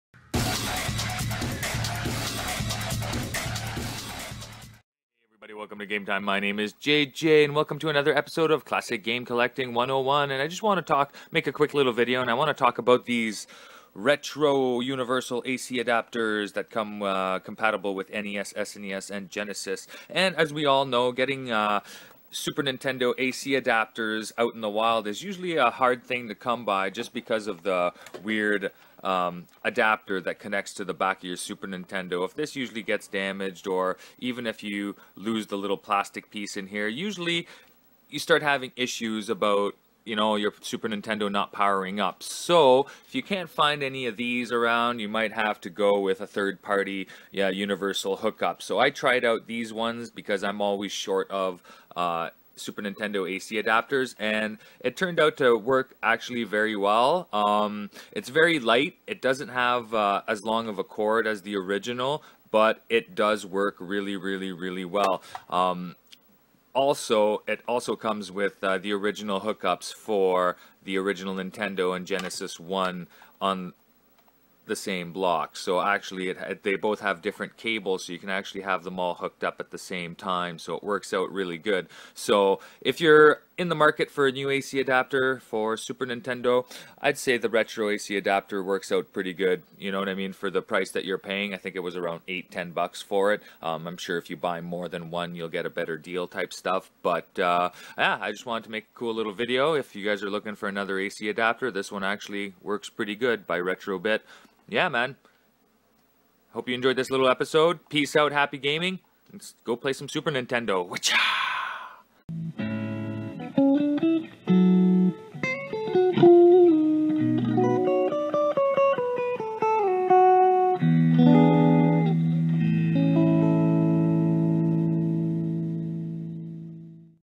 Classic Game Collecting #10 | Retrobit NES SNES GENESIS AC Adapter Review w/GameTime
In this episode I give you my thoughts on the Retrobit AC Adapter. Nothing worst than having a system and not having a power adapter. Sometimes you have to resort to a third party one so lets see how this one works. Enjoy and Happy Gaming